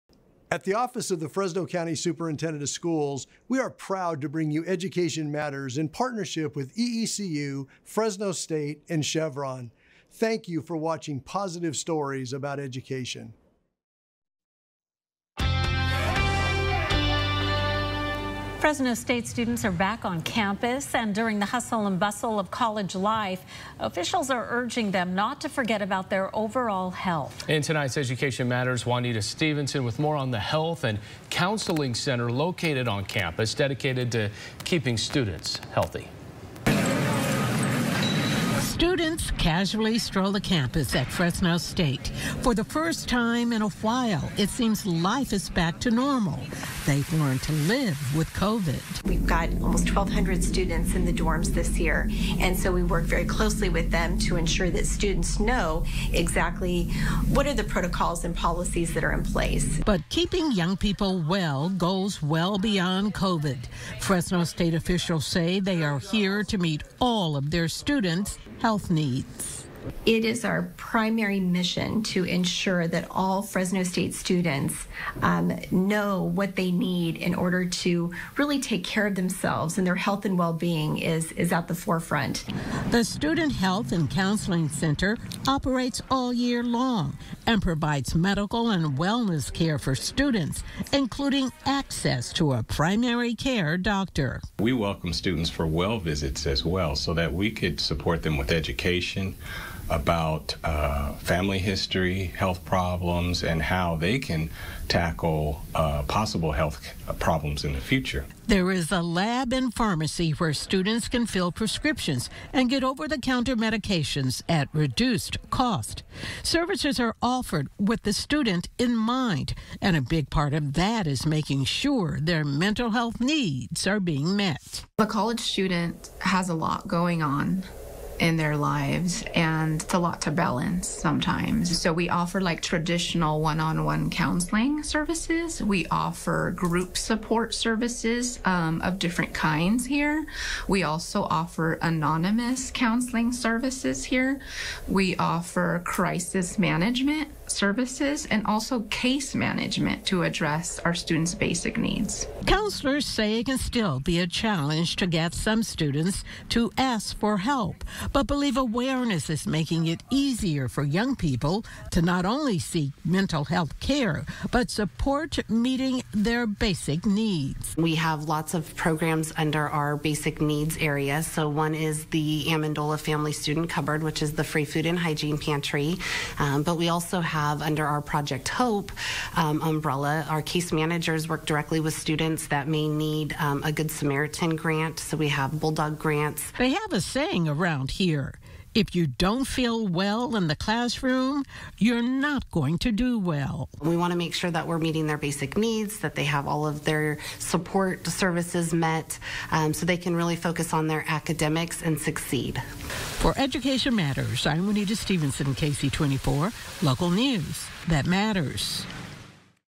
Fresno State Health Center
How Fresno State is ensuring the health and well-being of students as they return to campus this year.